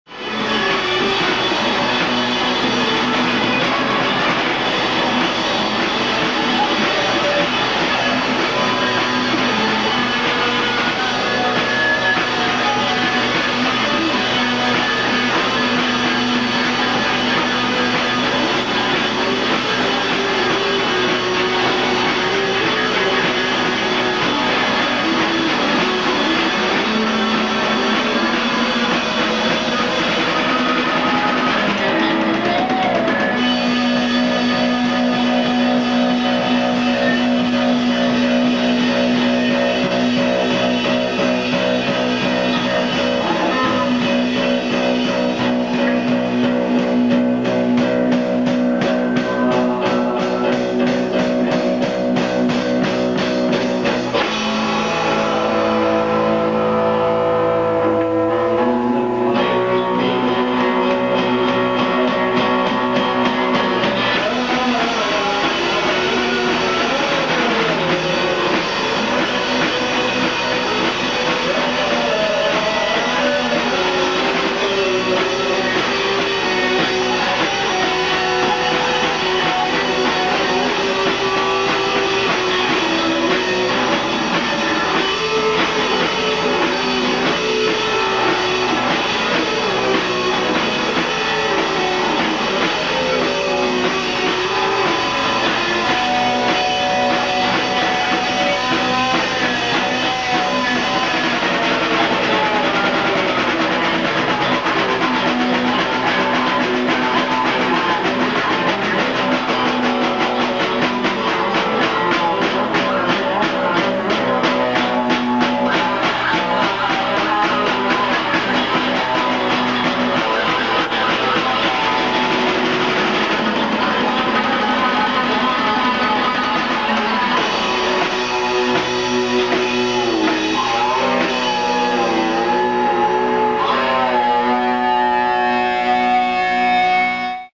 Plagal Grind (live) HD
Plagal Grind performing live somewhere in Dunedin, NZL in the 1990's.